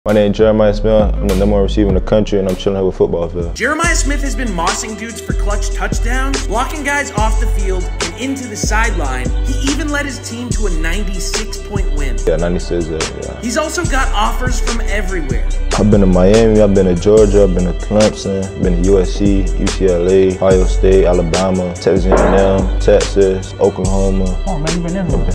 The #1 WR in the Country is a MENACE 😳
Jeremiah Smith is the #1 receiver in the country for a reason. (A lot of reasons)